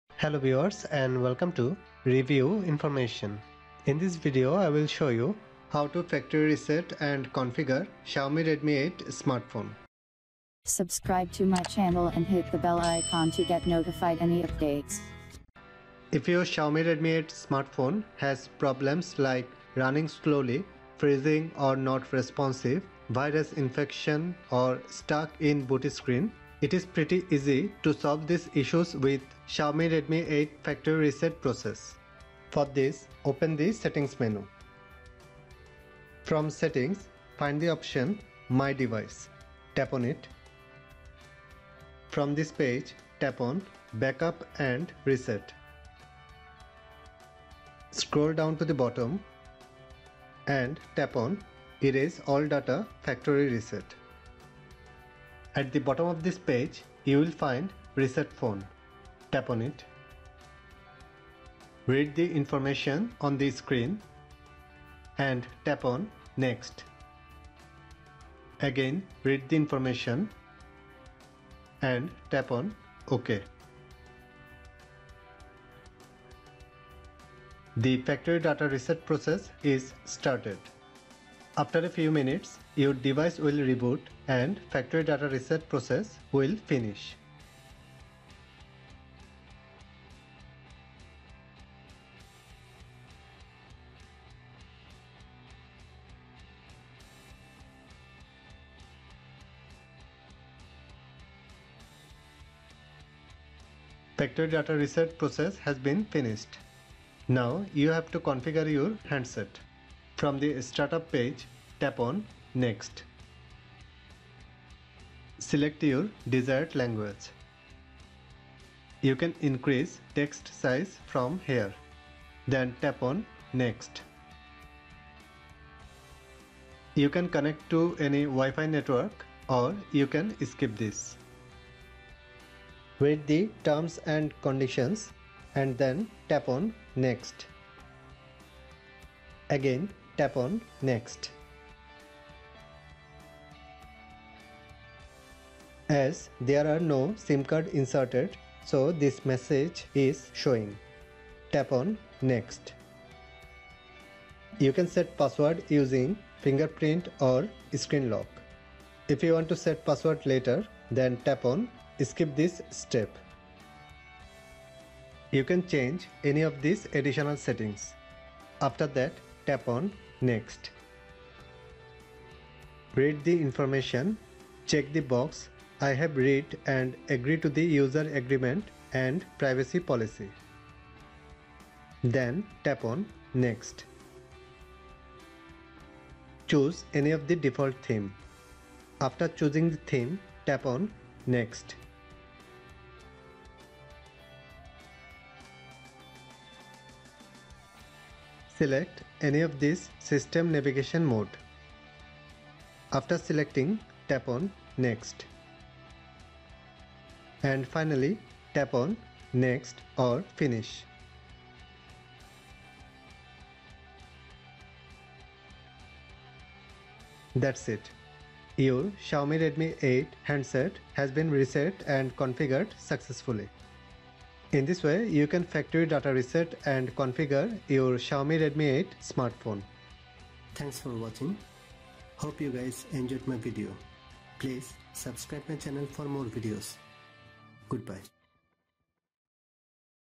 How To Factory Reset & Configure Xiaomi Redmi 8
In This Video I Will Show You, How To Factory Data Reset & Configure Xiaomi Redmi 8 Smartphones.